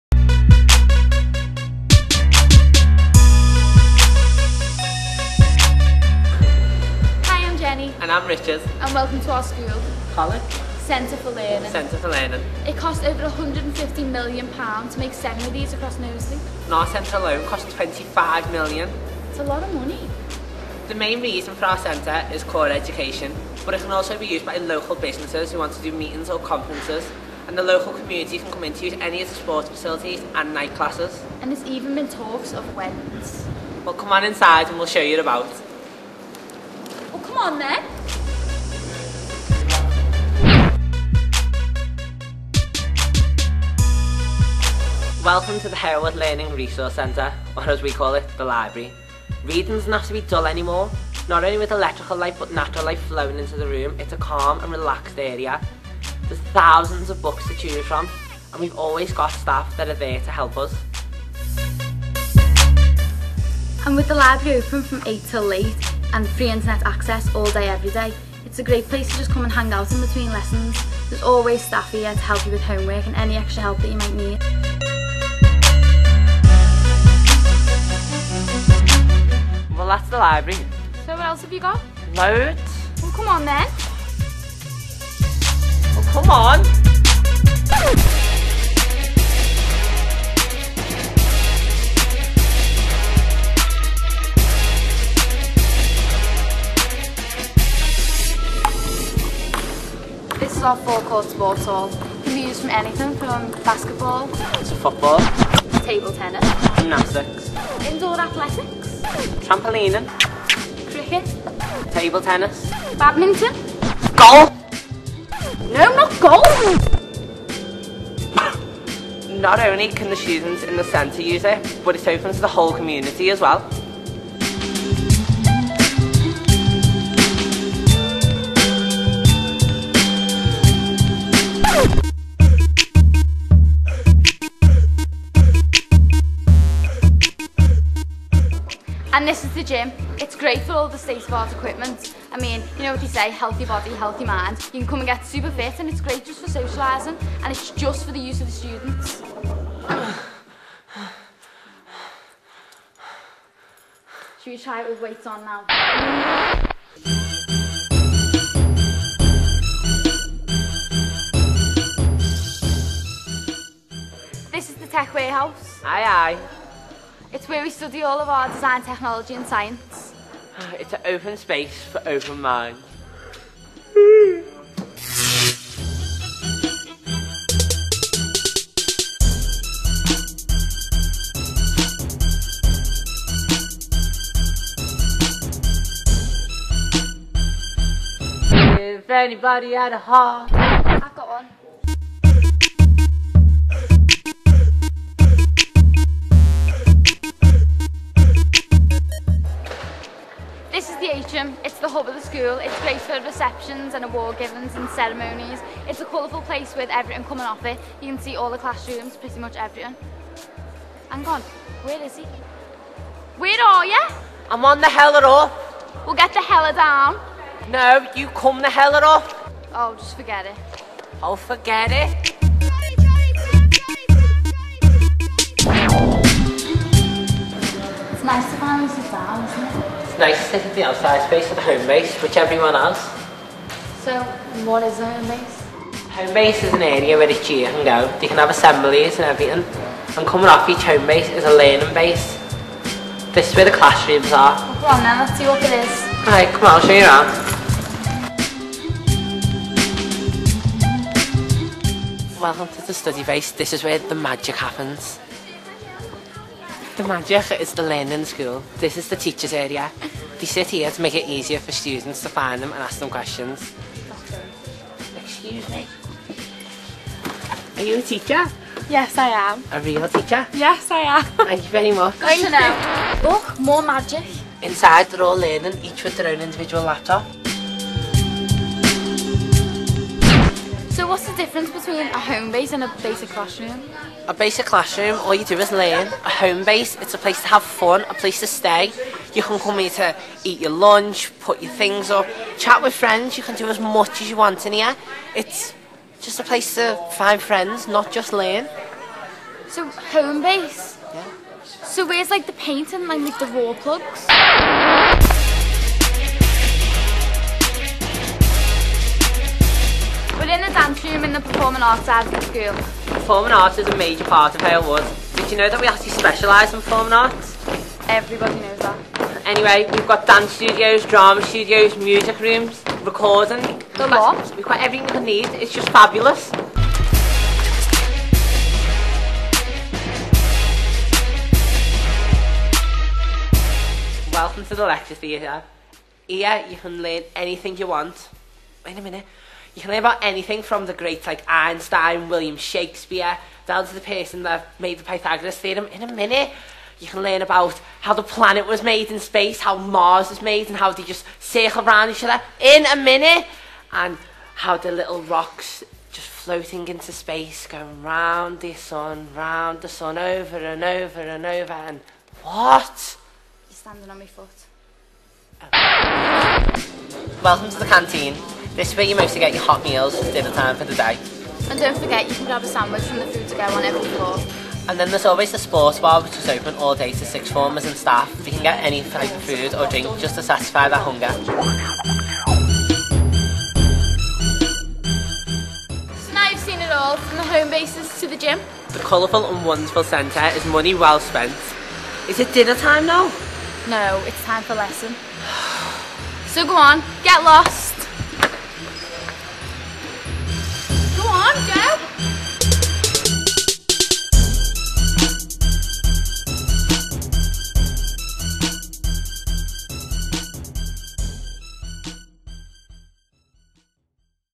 Halewood Centre for Learning - Cribs style
A look around Halewood's brand new Centre for Learning in the style of MTV Cribs.